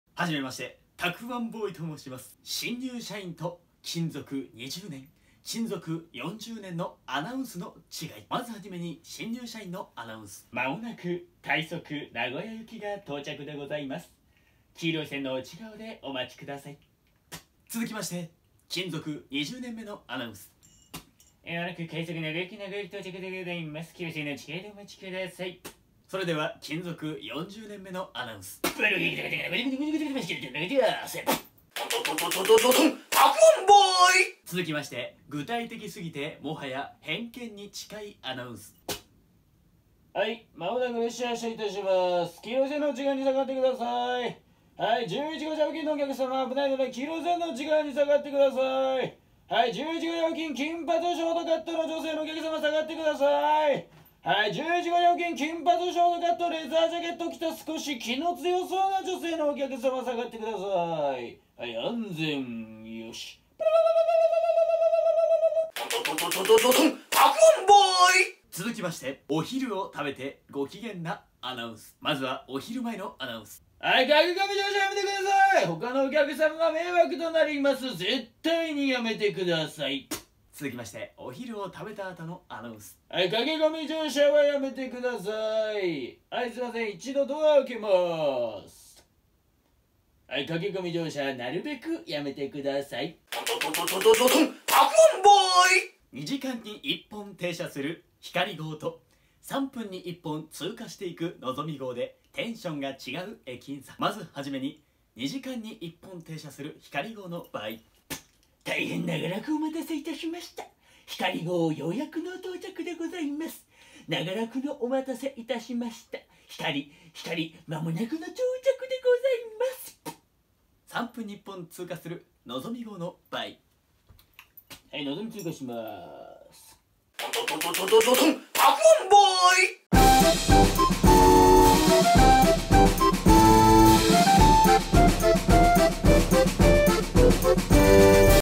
『駅員ものまね』ホームアナウンス編
皆さん駅のホームで何か思う事ありませんか？
元駅員の僕が今回は細か過ぎて伝わらなそうなホームアナウンスモノマネをやらせてもらっています。